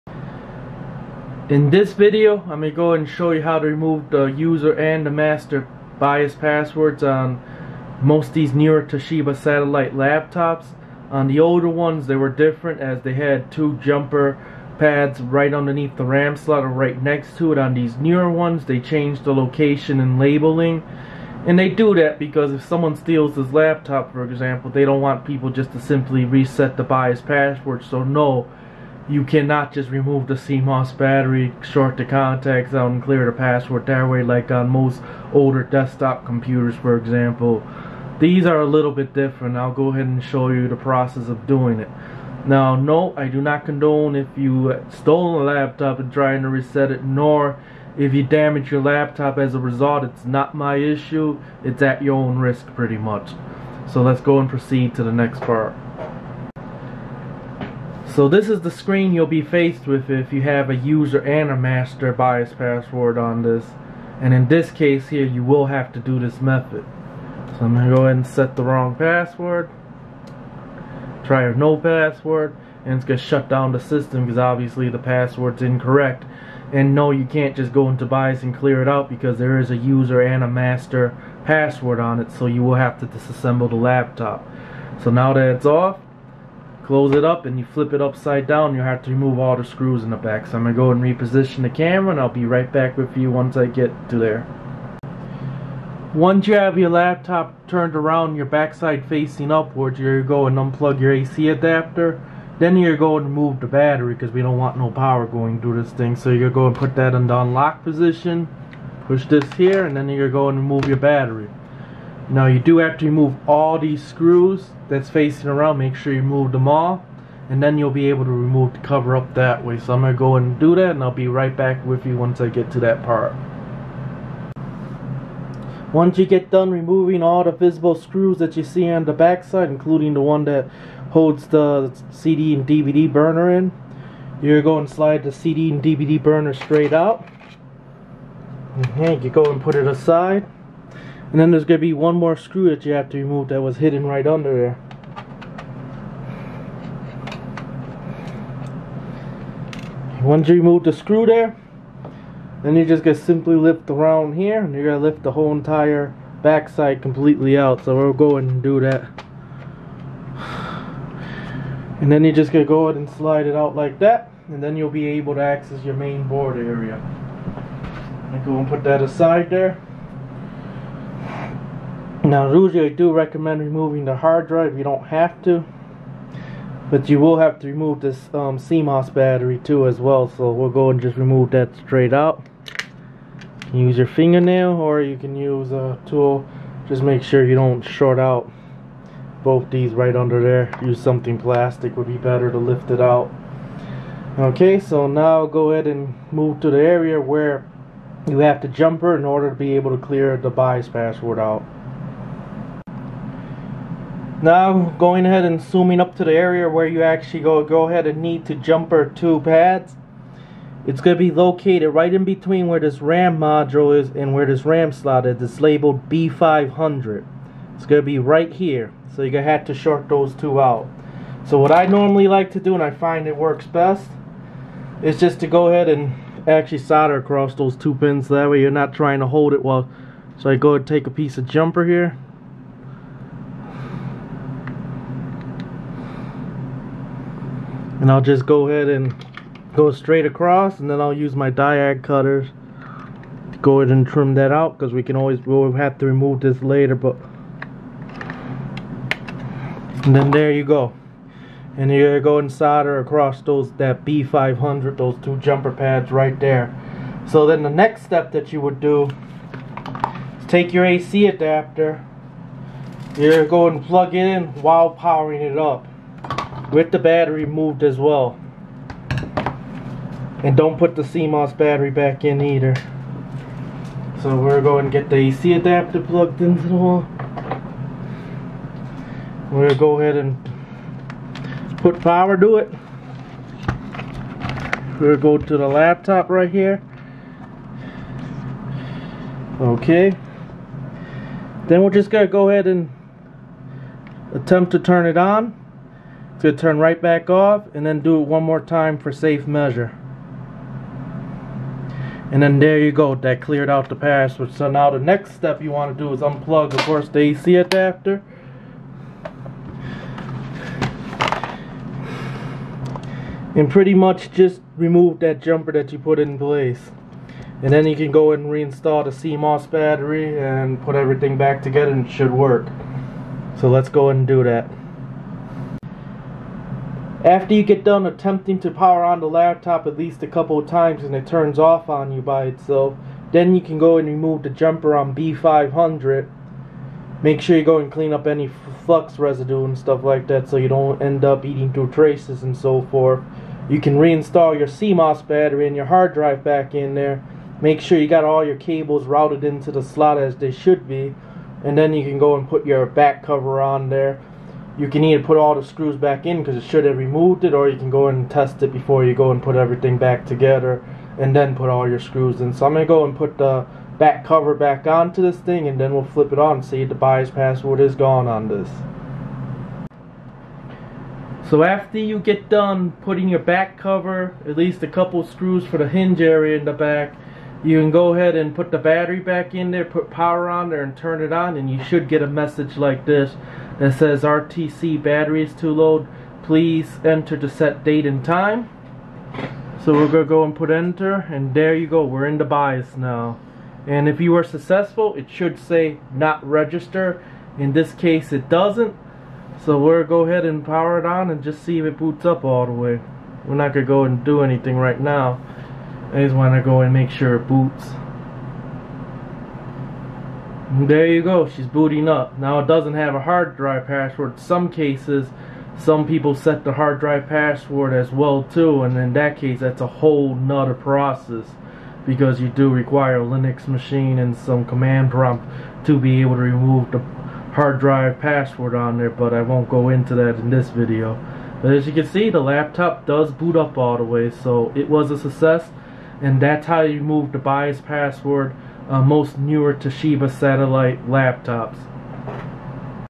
How To Reset BIOS Password On a Toshiba Satellite C75D B7230 Laptop
This is a video on how to reset a BIOS password on a newer Toshiba Satellite laptop with a B500 jumper pad on the motherboard next to WIFI and Ram module. Some models will vary on jumper location.